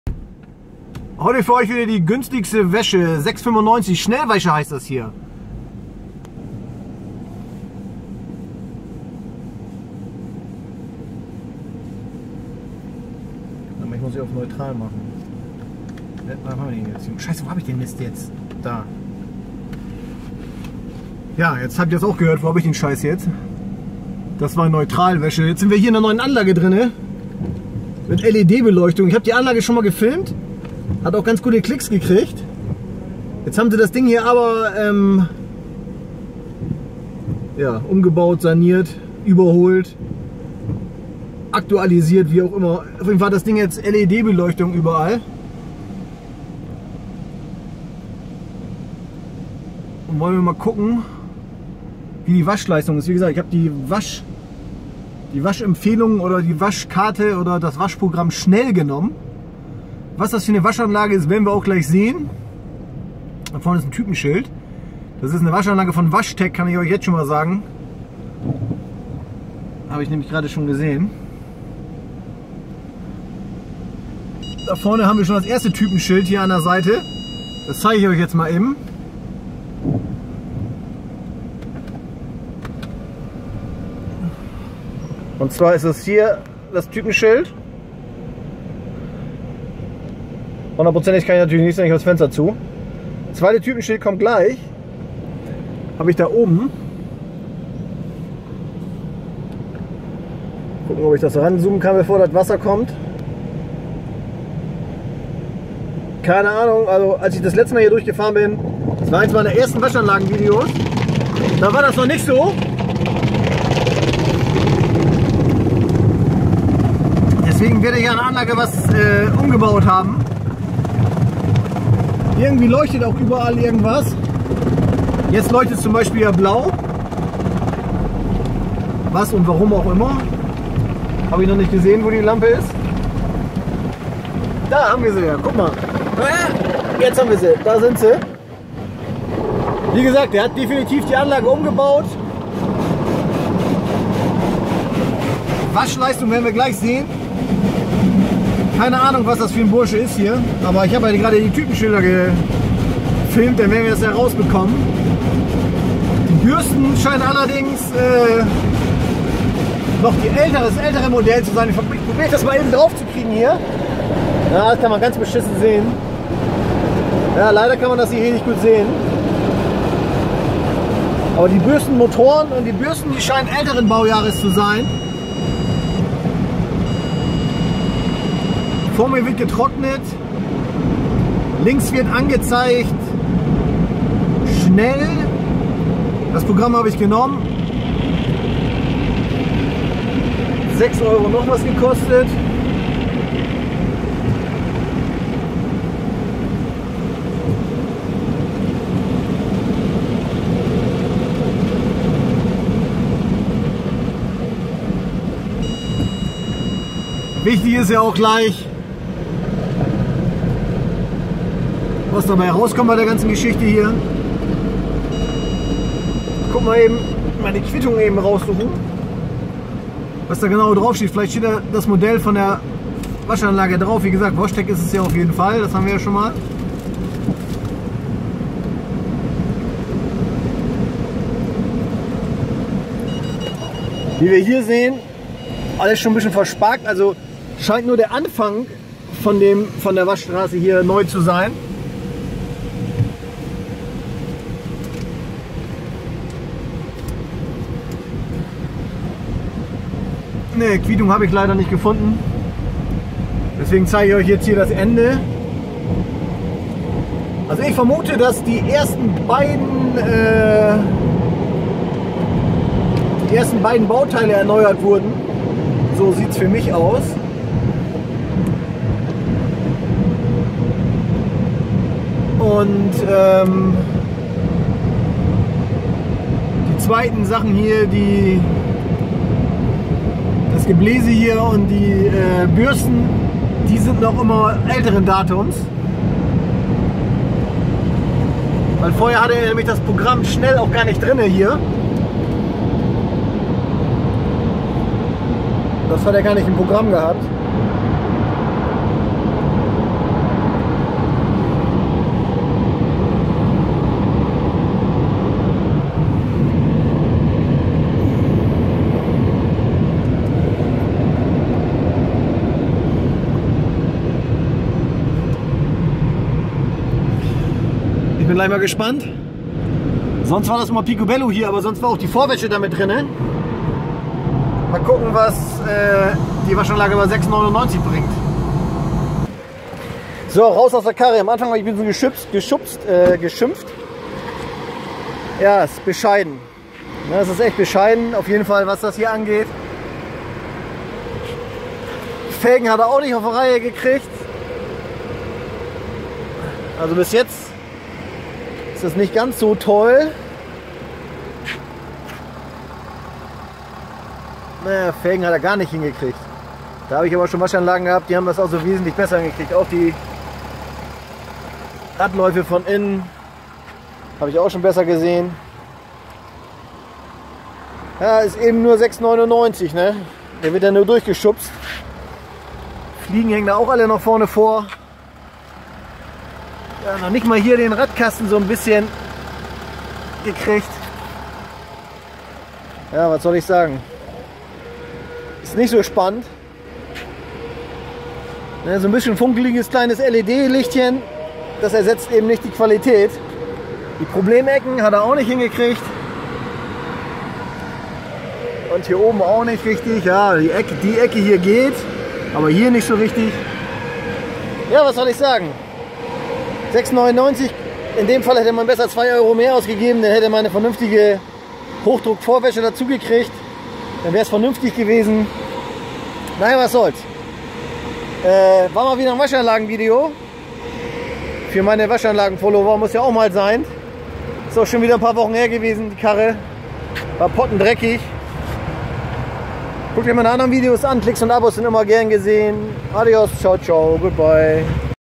WASH TEC WS 0540 SoftLine² - Waschanlage mit LED Beleuchtung 6,99 naja geht besser
Das war meine erste waschanlage hier auf YouTube Top mit über 31000 Klicks seht hier !!
Ja die hatte ich schon mal nu ist sie aber gepimt worden jetzt mit LED-Beleuchtung aber die Waschleistung bei dem schnell Pogramm lässt zu wünschen übrig .SoftLine²